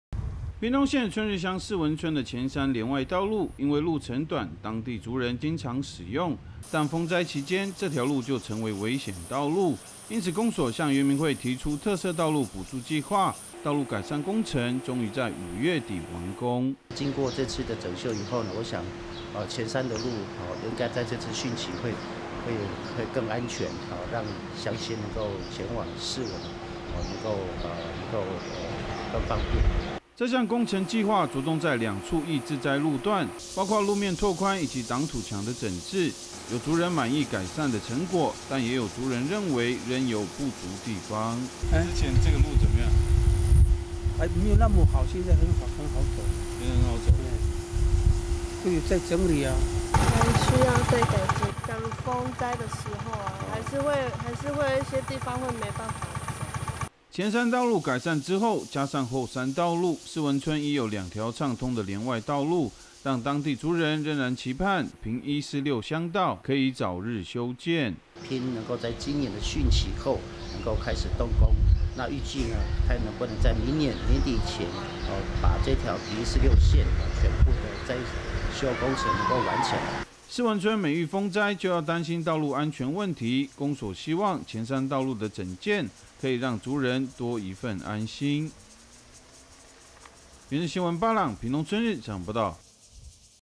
往士文村替代道路危險 改善工程上月完工 2019-06-09 IPCF-TITV 原文會 原視新聞
屏東縣春日鄉士文村的前山連外道路，因為路程短當地族人經常使用，但風災期間這條路就成為危險道路，因此公所向原民會提出特色道路補助計畫，道路改善工程終於在五月底完工。

這項工程計畫著重在兩處易致災路段，包括路面拓寬以及擋土牆的整治，有族人滿意改善的成果，但也有族人認為仍有不足地方。

前山道路改善之後，加上後山道路，士文村已有兩條暢通的連外道路，但當地族人仍然期盼屏146鄉道可以早日修建。

士文村每遇風災就要擔心道路安全問題，公所希望前山道路的整建，可以讓族人多一份安心。